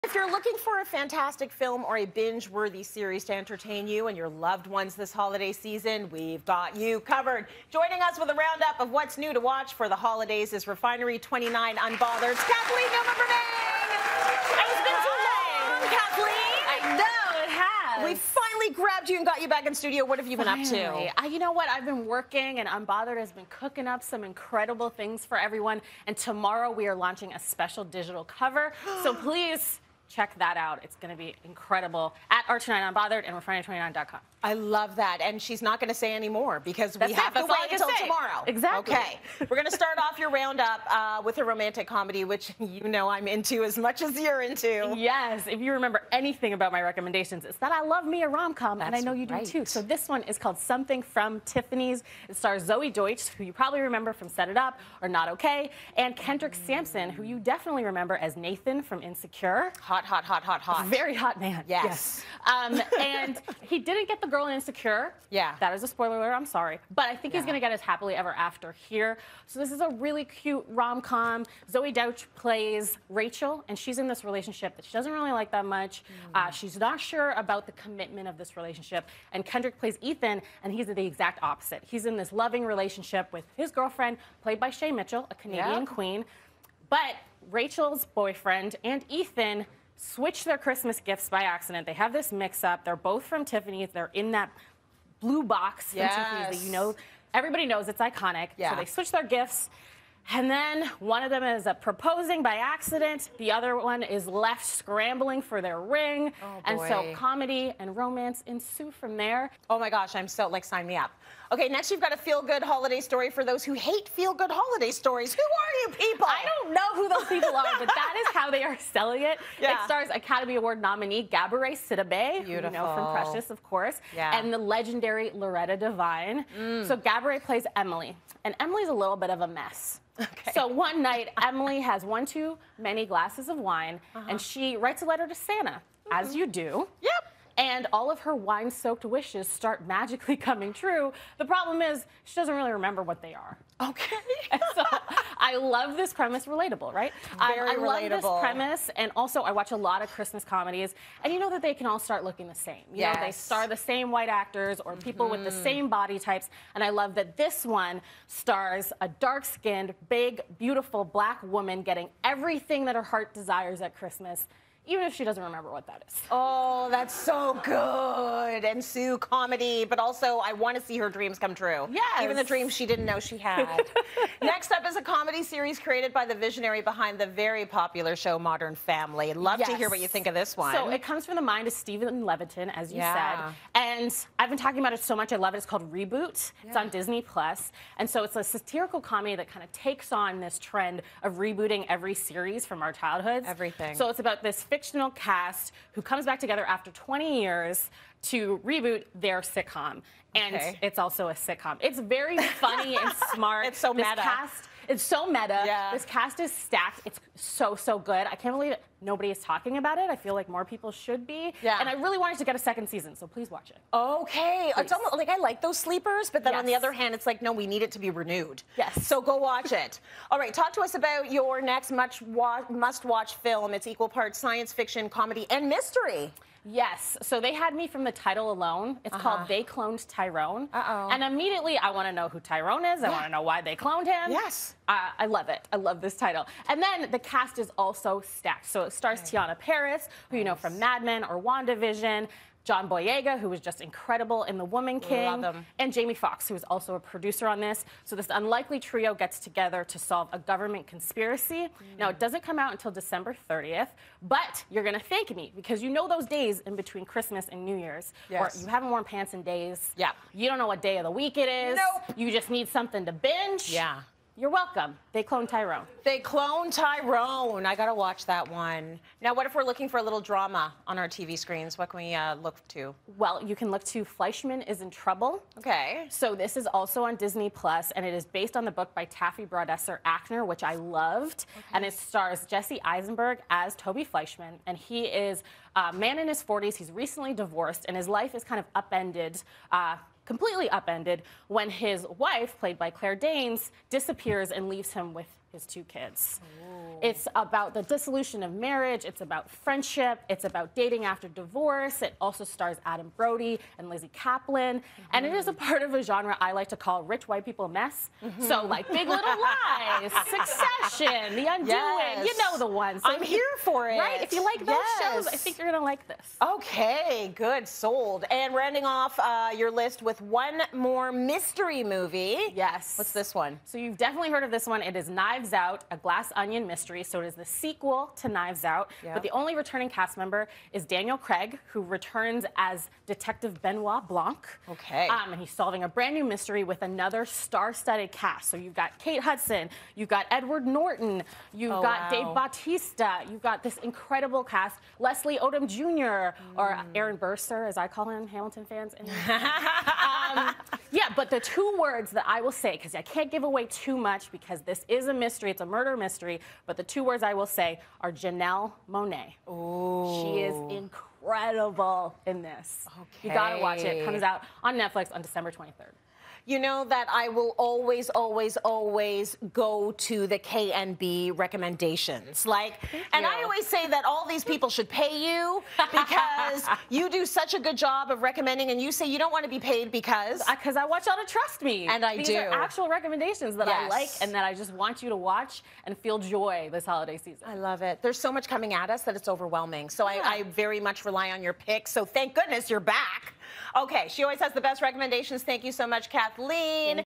6 new holiday films to watch this season
Pop culture expert Kathleen Newman Bremang rounds up the holiday content worth watching. "I love me a romcom and I know you do too!"